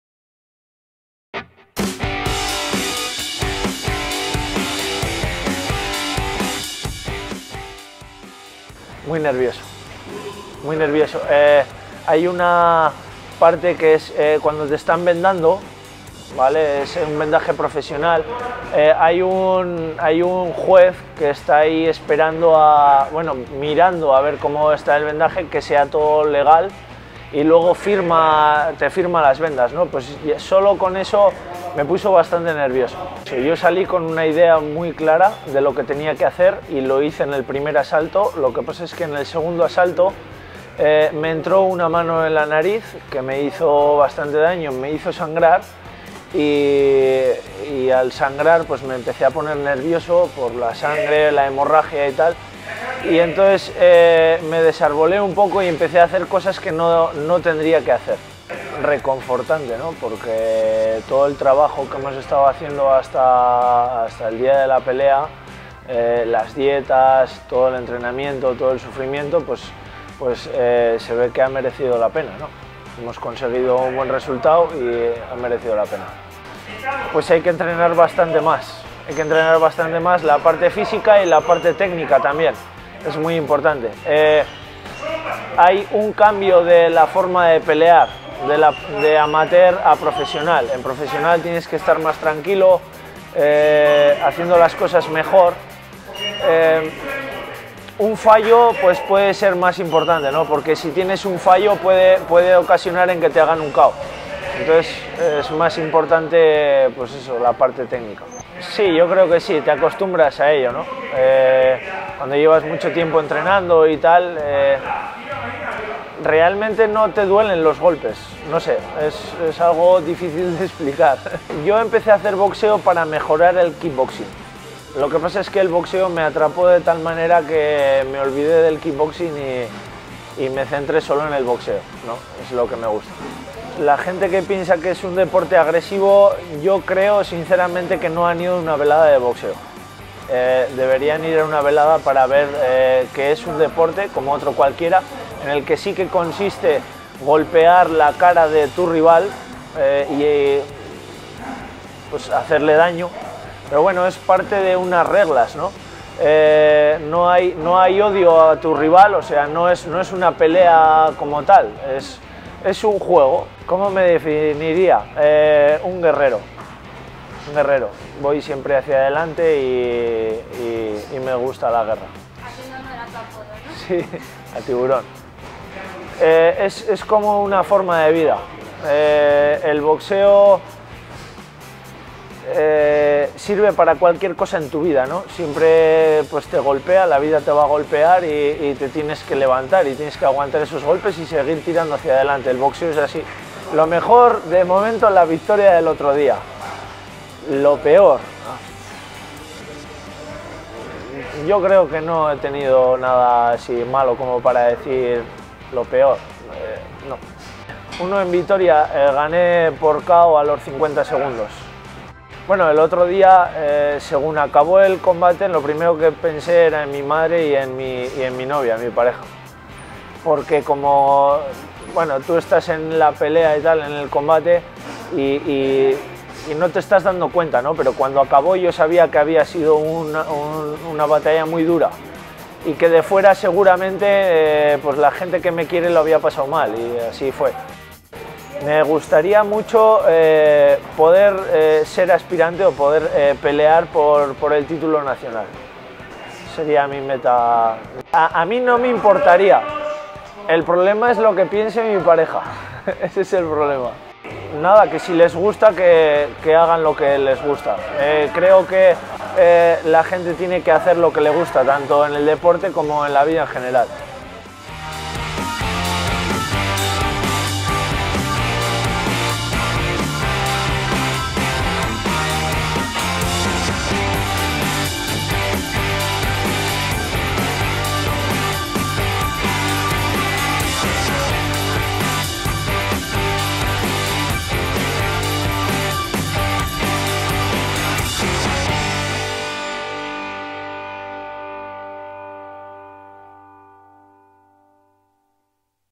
Un tiburón con guantes de cuero.
Reportaje sobre el boxeador Jorge Vallejo, el `Tiburón´de Oyón.